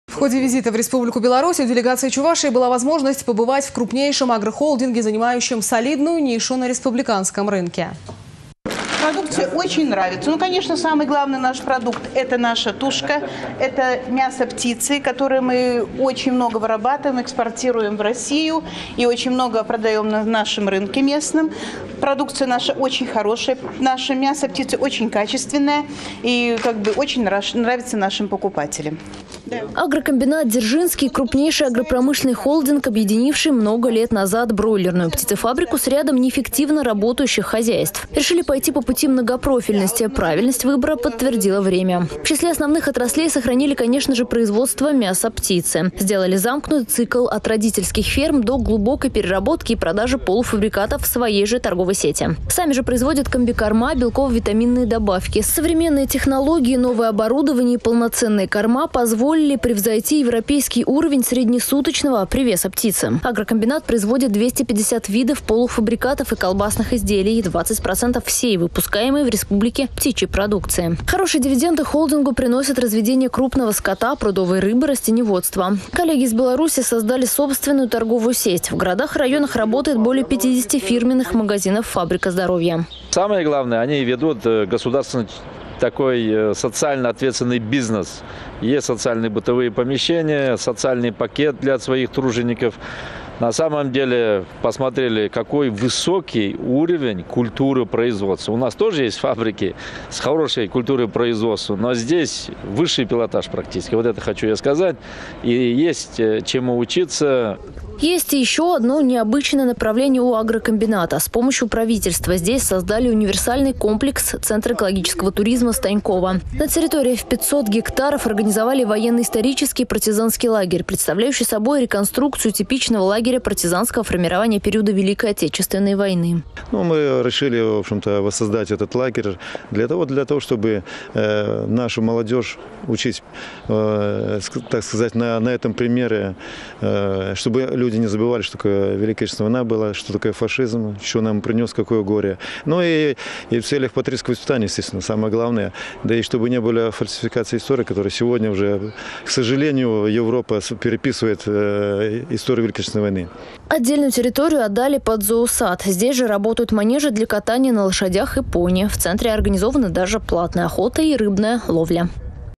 Делегация Чувашии посетила агрохолдинг "Дзержинский"
В ходе визита в  Республику Беларусь у делегации Чувашии была возможность побывать в крупнейшем агрохолдинге, занимающем солидную нишу на республиканском рынке